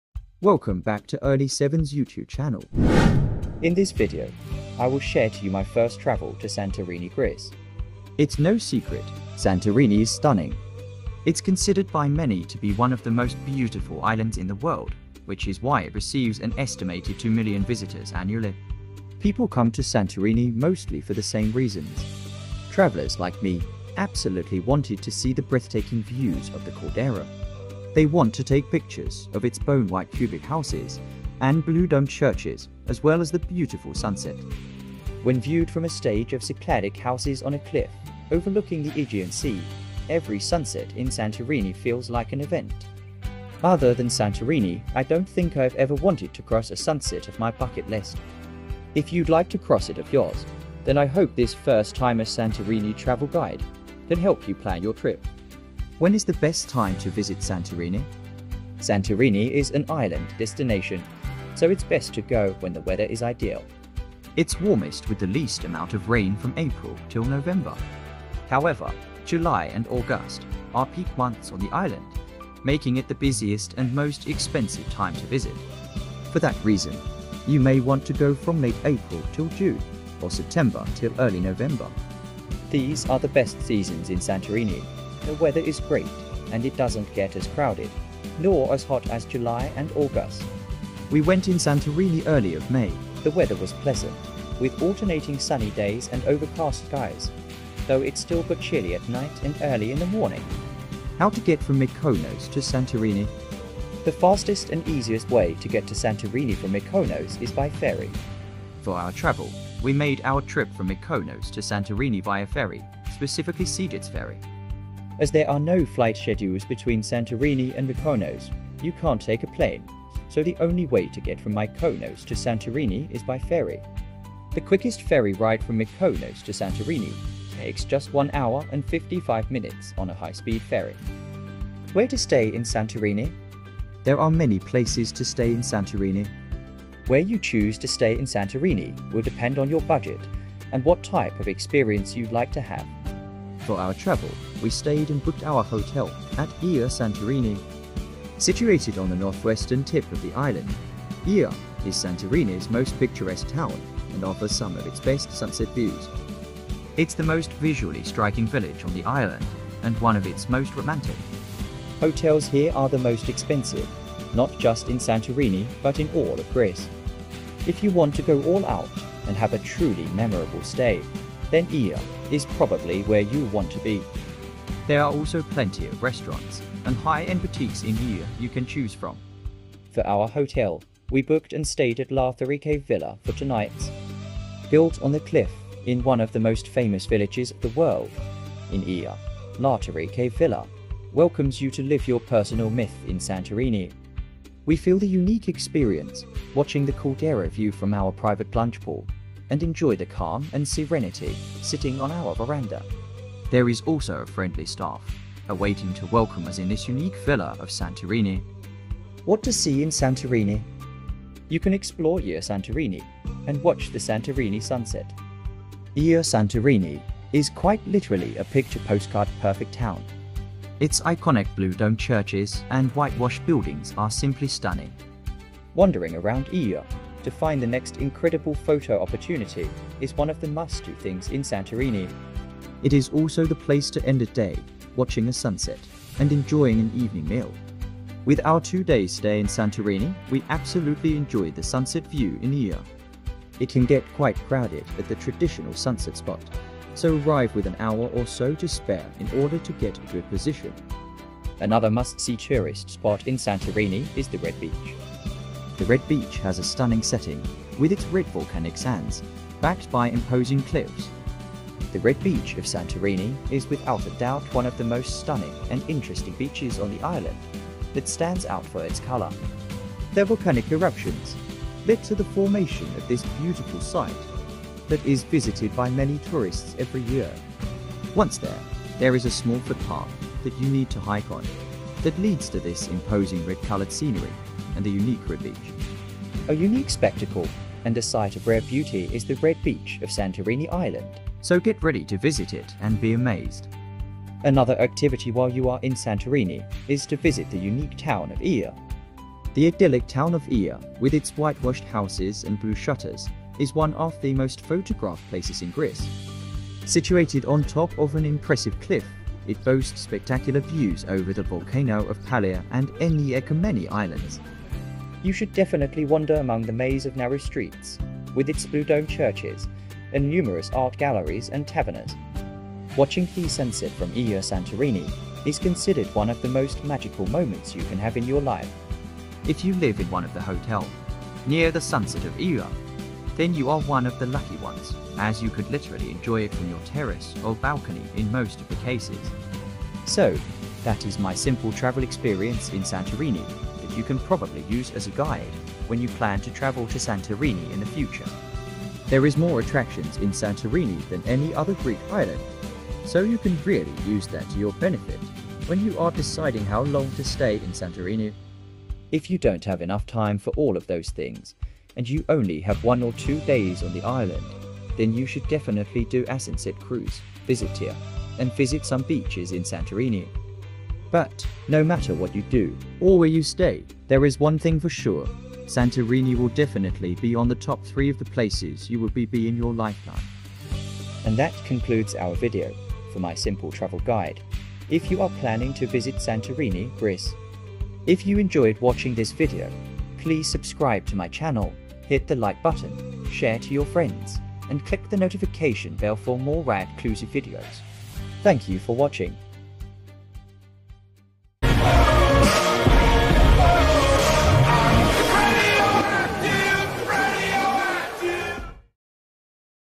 ULTIMATE TRAVEL GUIDE TO SANTORINI || Where to Stay and How to Get to Santorini
Travel Guide to Santorini Greece

In this video, I will share to you my travel experience in Santorini, how to get from Mykonos to Santorini, where to stay and where to go around Santorini. Greece is one of my bucketlist specially santorini, the blue and white signature of structural buildings and picturesque view of caldera.